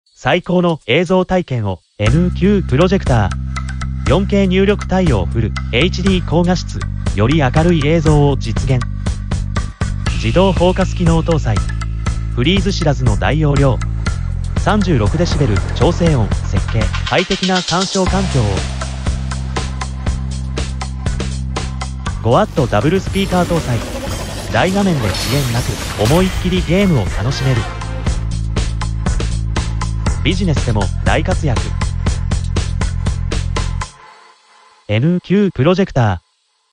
お家で極上のシアター体験を！ 4K入力対応＆フルHD高画質「N9」プロジェクター
フルHD の高い解像度で、大画面180インチの映像をお楽しみいただけます。映像だけでなく、サウンドも妥協しません。2つの5Wスピーカー搭載で、立体感ある高音質をご提供します。N9プロジェクターで、ご自宅の一室がシアタールームに！